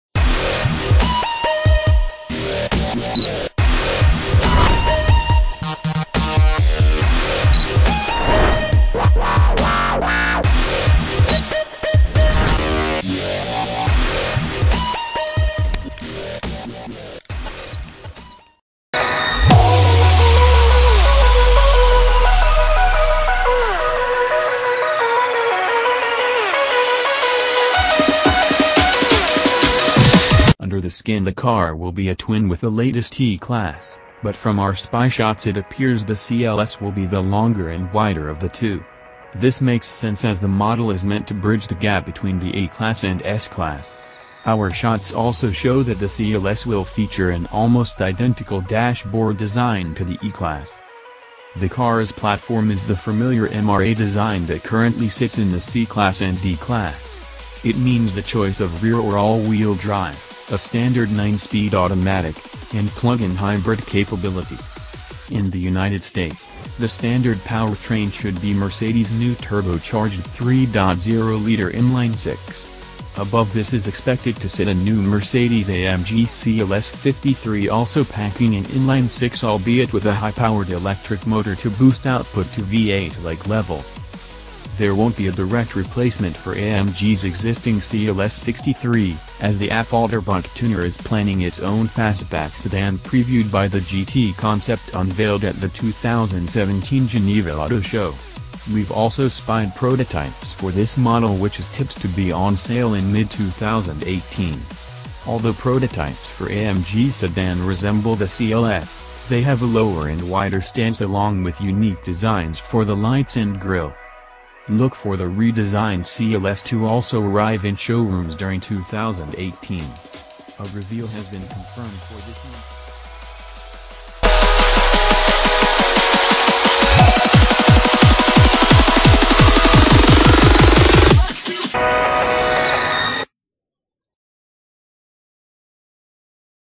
LOOK THIS!!NEW 2019 Mercedes Benz CLS Interior Exterior Sexiest Mercedes ALIVE
LOOK THIS!!NEW 2019 Mercedes Benz CLS Interior Exterior   Sexiest Mercedes ALIVE -Under the skin the car will be a twin with the latest E-Class, but from our spy shots it appears the CLS will be the longer and wider of the two. This makes sense as the model is meant to bridge the gap between the E-Class and S-Class. Our shots also show that the CLS will feature an almost identical dashboard design to the E-Class.

Popular Post !